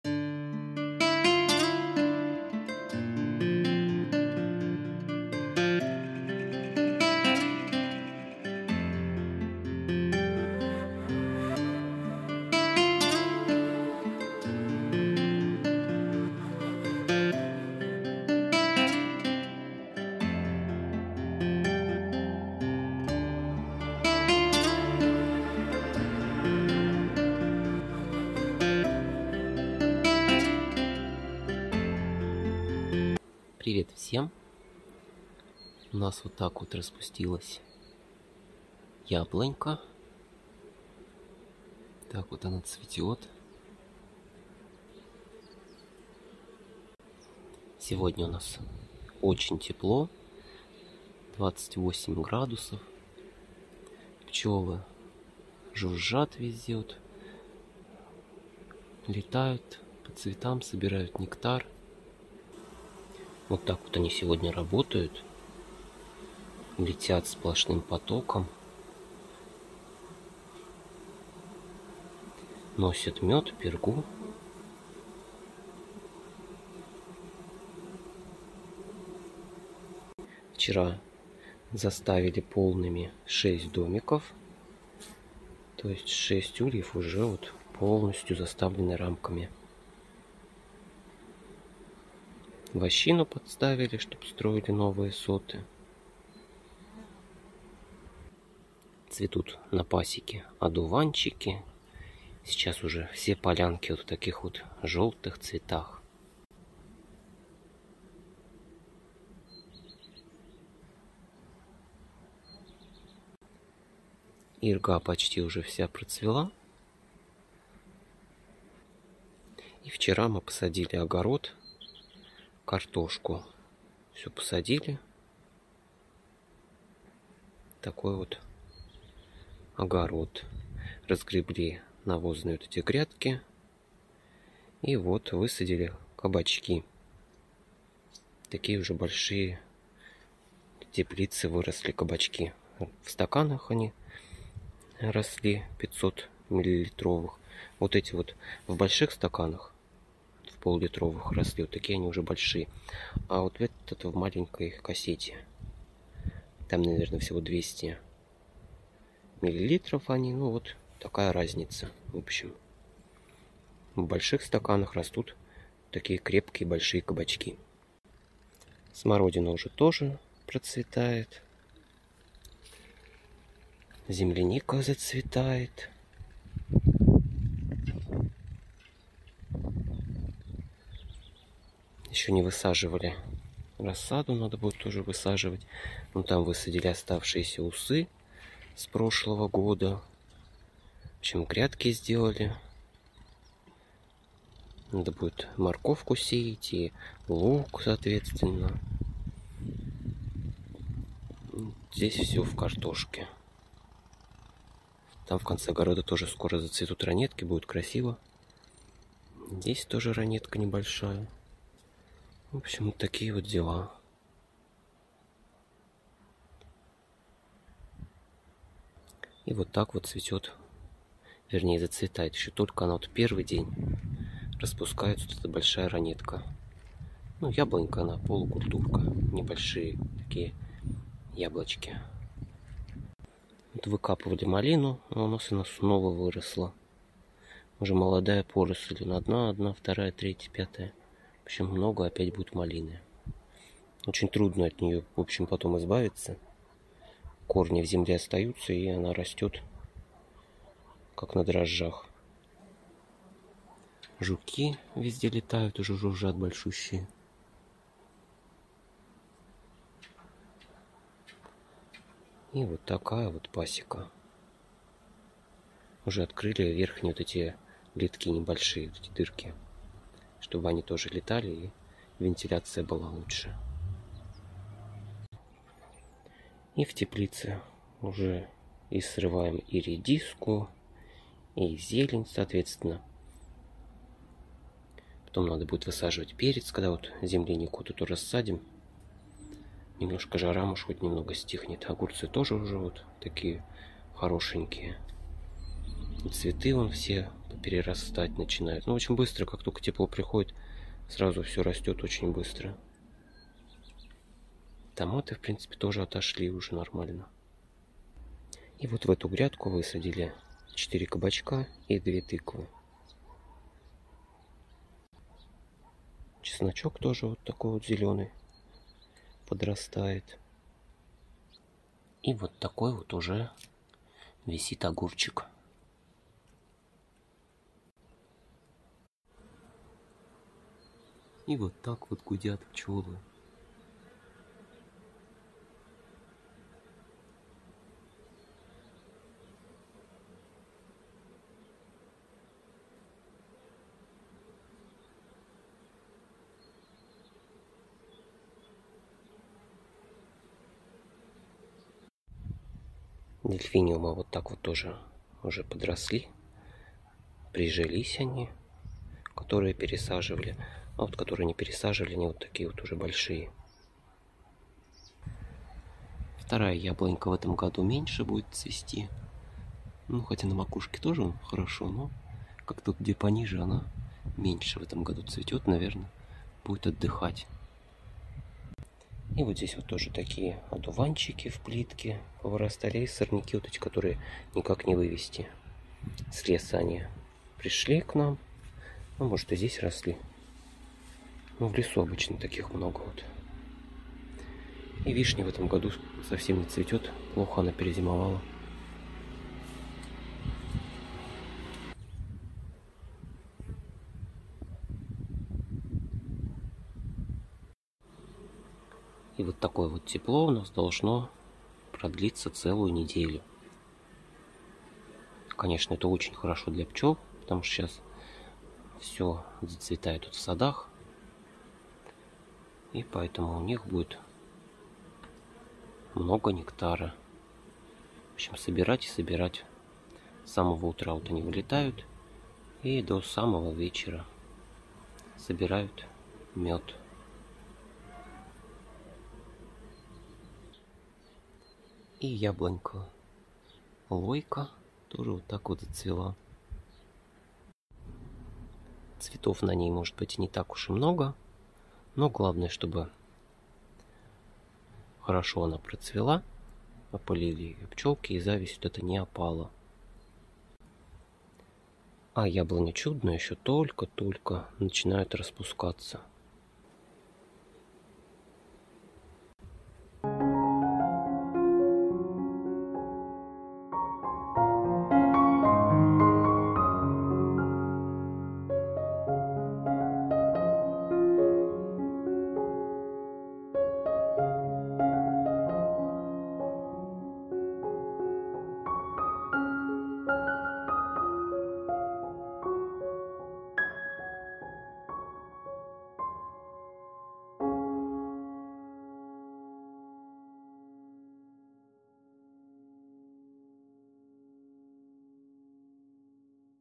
Цветущий яблоневый сад. Посадили картошку и сделали грядки для посевов.
Наши яблони цветут, много красивых одуванчиков, пчелки носят мед. Посадили картошку, приготовили грядки для посевов овощей и других культур.